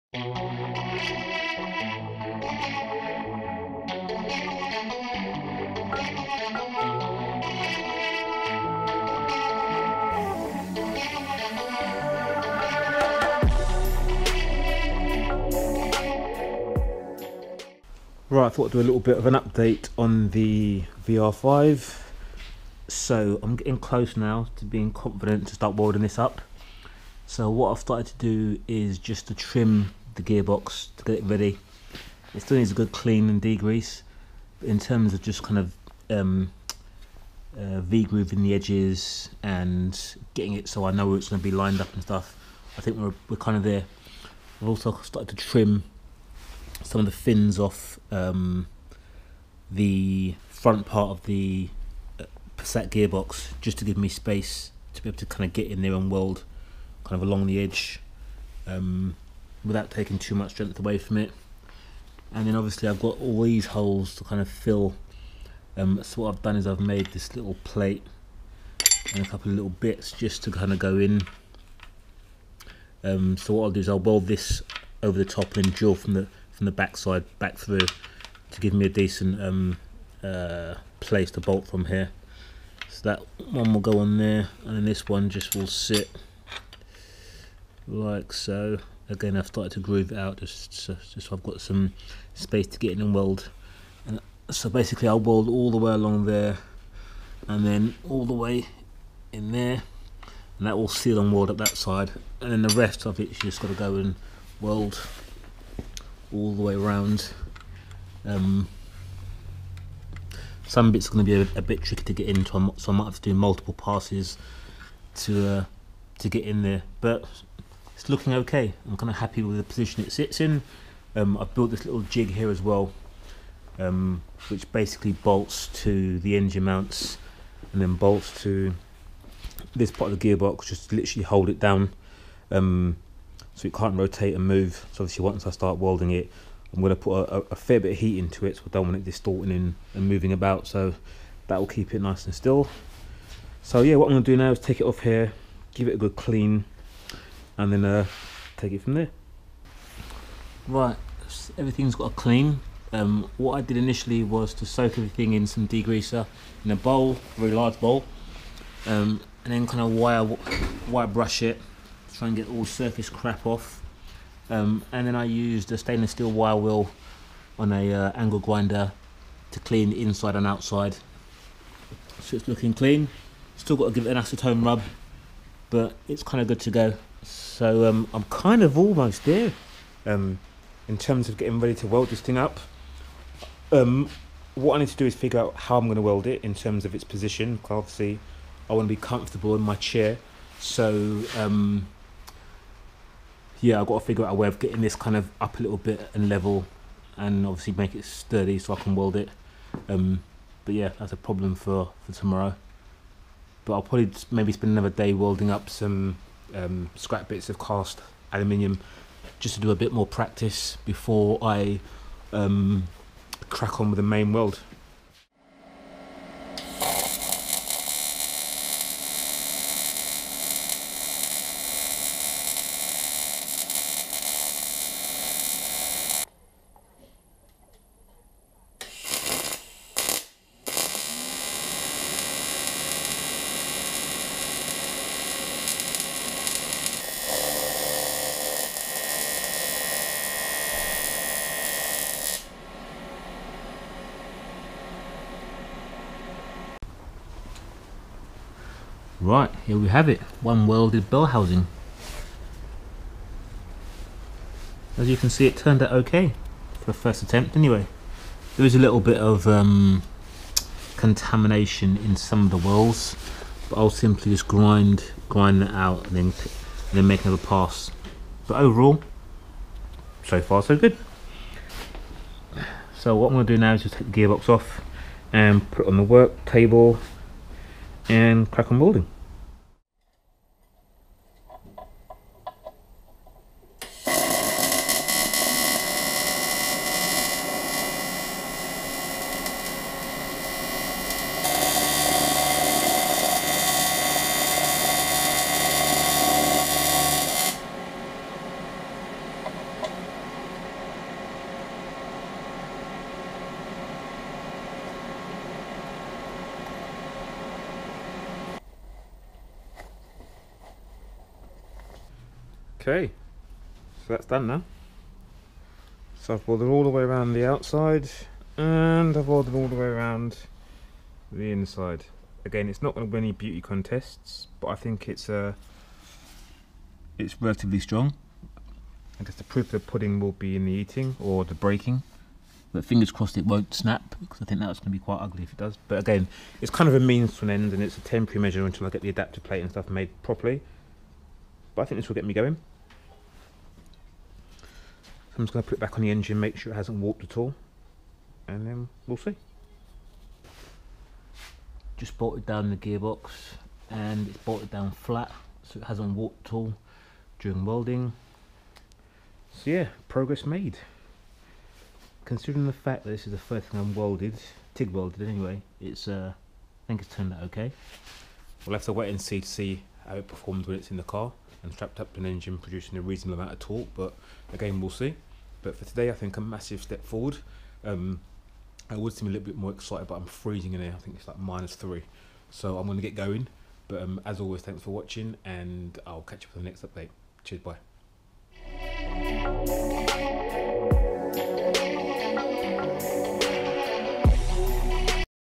Welding a ZF Bellhousing / BMW Gearbox
Update on the BMW VR5 Turbo conversion.  TIG welded the bellhousing

Intro & Outro:
Kid Zolt
Guitar track